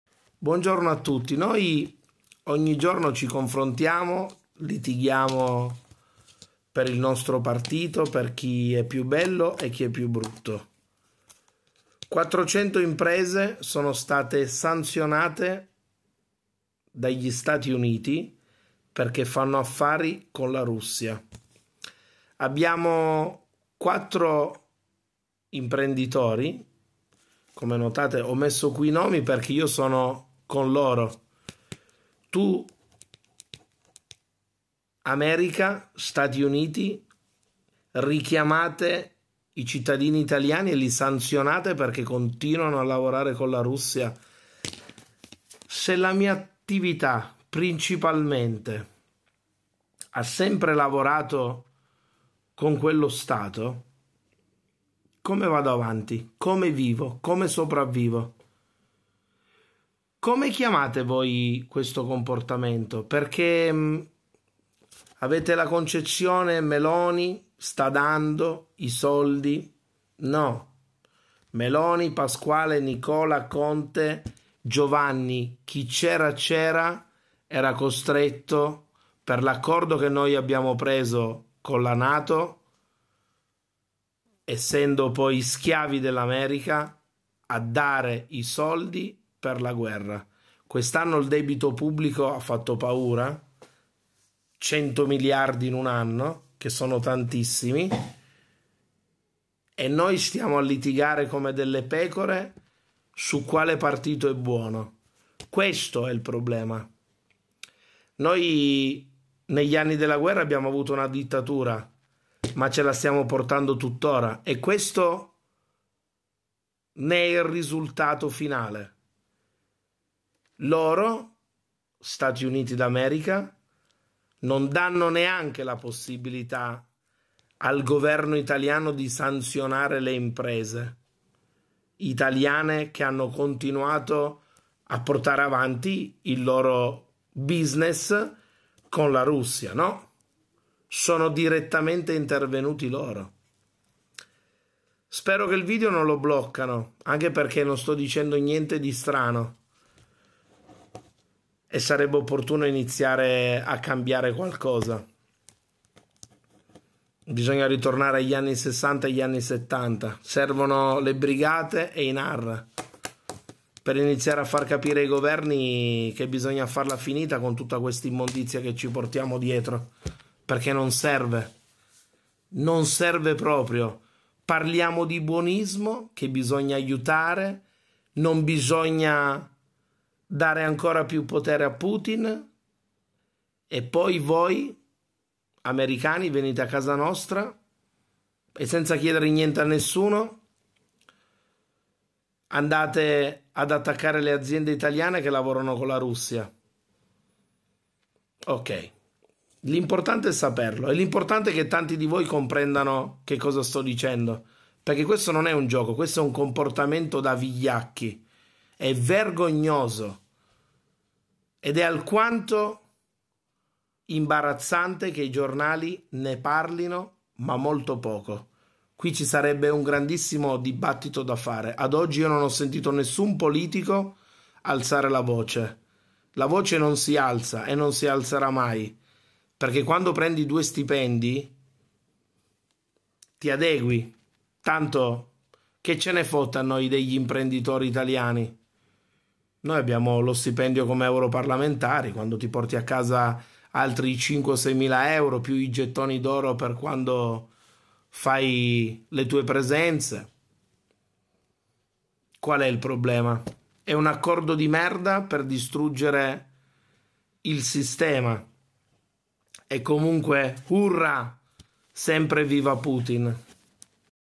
Usa sanziona imprenditori italiani
uno stato che non fa parte della comunità Europea che sanziona imprenditori italiani perché lavorano con la Russia.